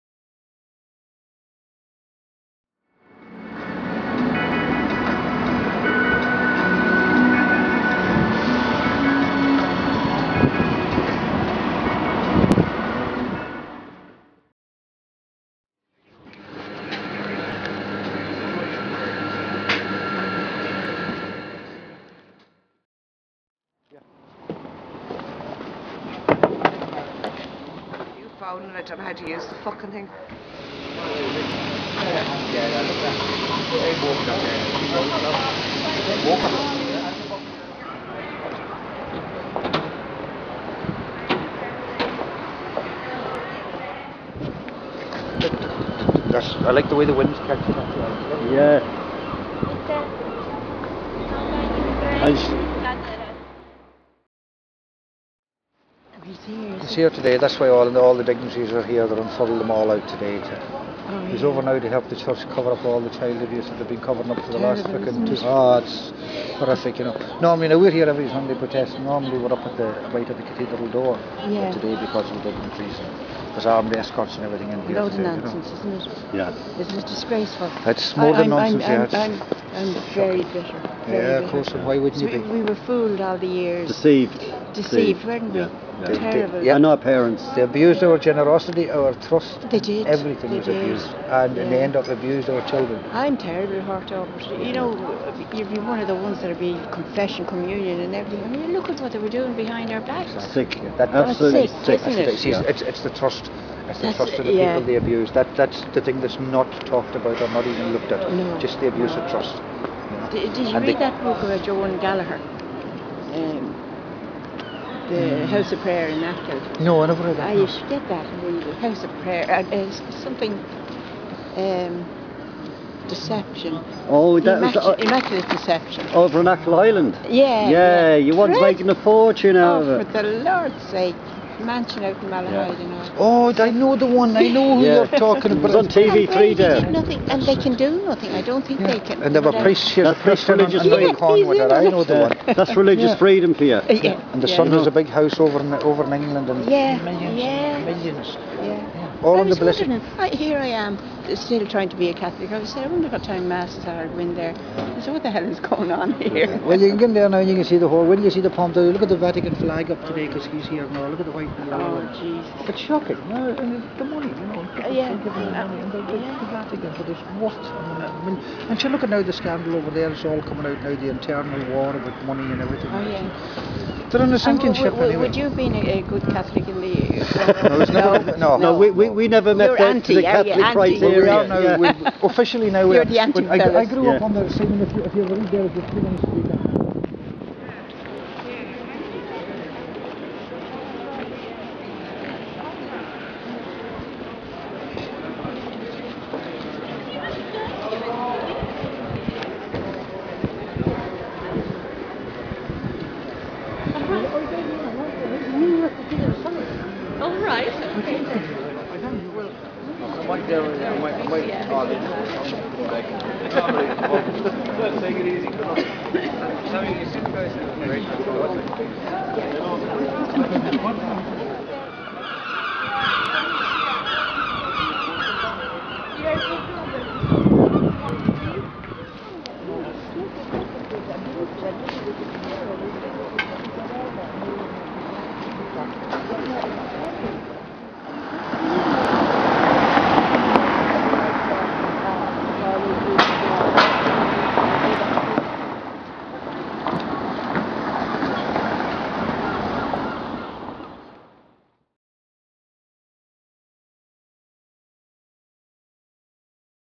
ACCAI PROTEST - 1st ANNIVERSARY - PAPAL NUNCIO COMES TO TOWN (1)
Today was ACCAI's first anniversary. Through out 2011 ACCAI protested at the Pro-Cathedral against the church in it's abuses on women, children who suffered horrifically.  The children of Ireland were caught up in a nightmare no child or adult should have had to suffer. Children are innocent.  The church have contested every allegation against it, delaying many cases up to 18 years.  The scale of abuse is breath taking, scanning the whole country, very few escaped  abuse through childhood. Today the church is going on all out for the PR that the church is cleaned up and all is well.
Many from the industrial schools/orphanages who went to the redress Board were silenced in the oath they had to sign declaring they could never reveal their case.
The catholic church can never move on while it leaves those it damaged out in the cold and with out Justice.  The Magdalenes and Mother and Baby Homes were used for child trafficking and slavery of innocent women.  These ladies and their families have never been addressed by the church or government .  Children were signed into Adult Mental Institutions by clergy and left there for the remainder of their lives. Children were dissected in college's, sent by the nuns from the orphanages and Mother and Baby homes. Children who didn't survive through force feeding or unauthorized vaccine trials were sent for dissection.
Priests raped children at will and were in no danger of ever being caught.  They were moved on when ever they came to the attention of the bishops of Gardai.  The perpetrators were guarded and protected at all times.  Allegations were always denied regardless of the severity of the crime.  Clergy that were  moved around also had their name's changed anywhere they were sent making it impossible to trace.  Every obstacle has been placed in front of victims/survivors to delay and erode away time.  The Ferns, Murphy, and Cloyne reports all cover the sexual abuse of children. The Ryan report illustrated the Institutions, a separate issue from the other reports of clerical abuse.  The Ryan report  involves the church, State and government, the roles they played and the extent of the damage done to children.

The cover up was as wide spread as the abuse.  Church,State or Government have never been brought to account for decades of abuse on the Irish people.

The children of Ireland were never and still are not cherished or treated equally before the law.